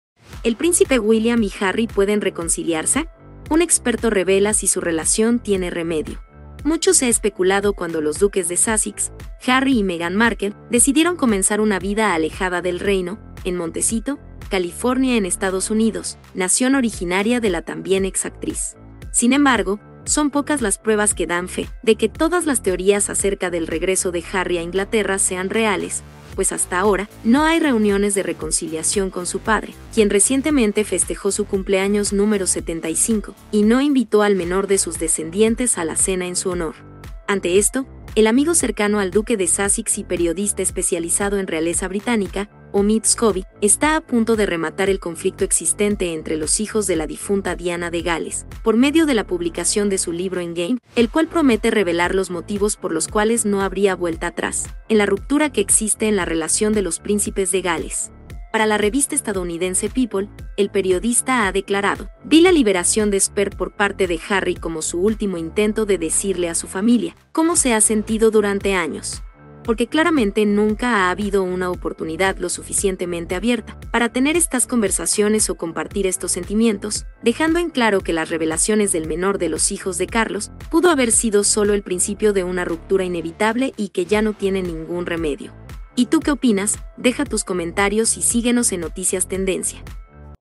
¿El príncipe William y Harry pueden reconciliarse? Un experto revela si su relación tiene remedio
El especialista en temas relacionados con la Familia Real británica Omid Scobie hace fuerte revelaciones acerca lo que viene para los hijos de Carlos III y Diana de Gales